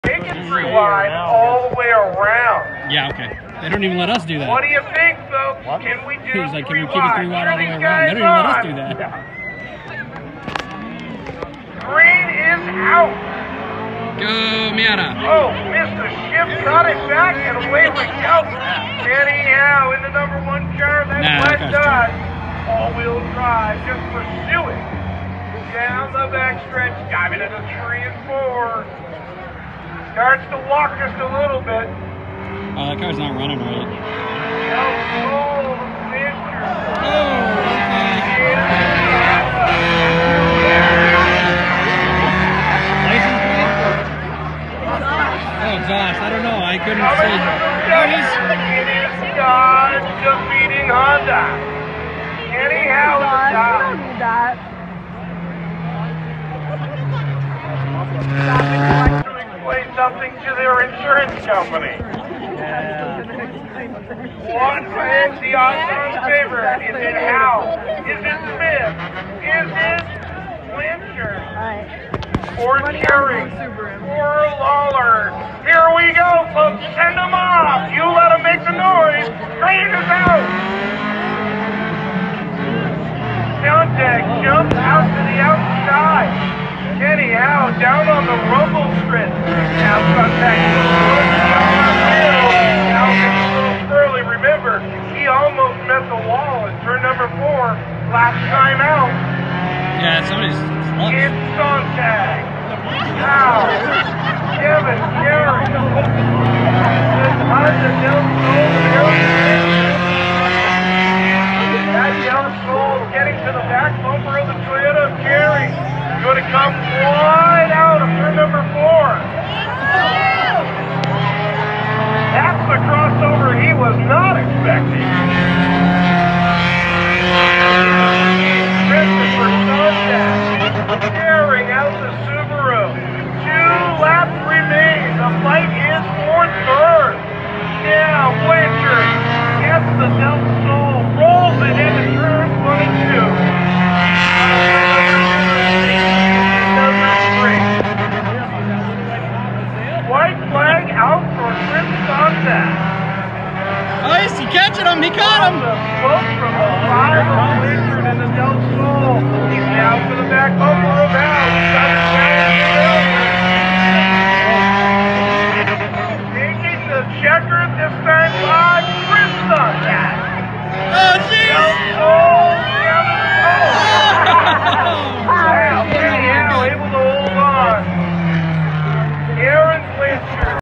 Two Spectator Races at Monadnock Speedway 9/17/22
Two of the races that Monadnock Speedway where people (must have helmet) take their streetcars on the track.